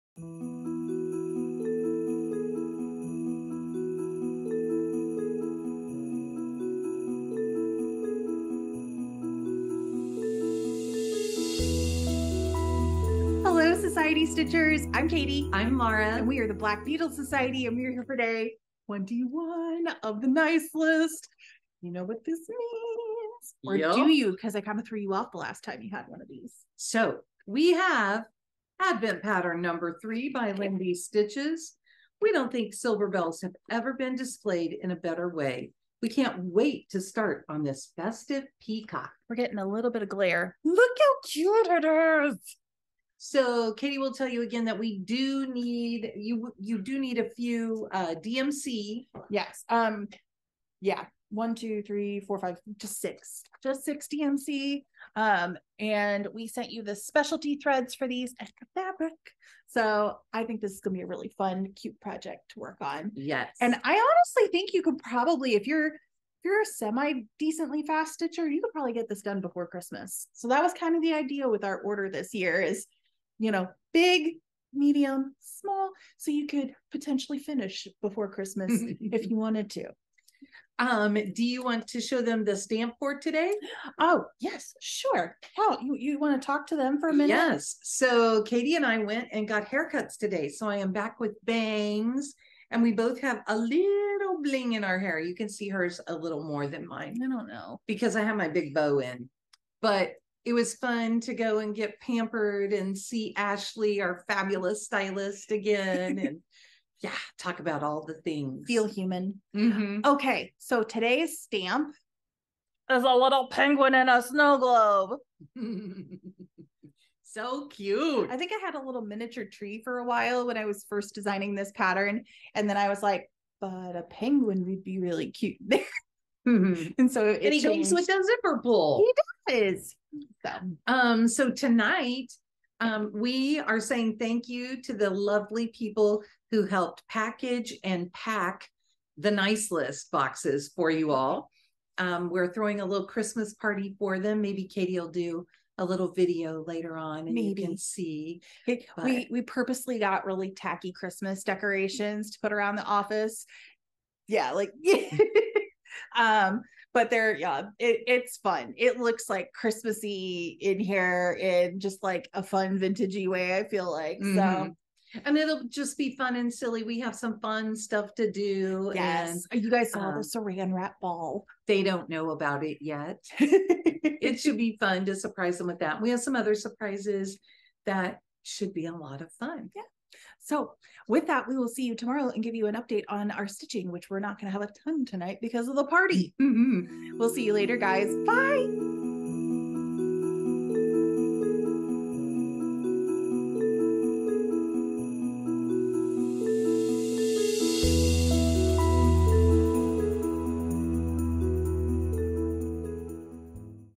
The Black Needle Society FlossTube— 12DaysOfStitching Day 21
Hello Society Stitchers!
Throughout the month of December, we will be sharing our stitching progress for 12DaysOfStitching hosted by The Black Needle Society here on FlossTube, and on Instagram! We will also be sharing unboxings of our The Nice List Box throughout the month as we show you our stitching progress as well. Happy Stitching!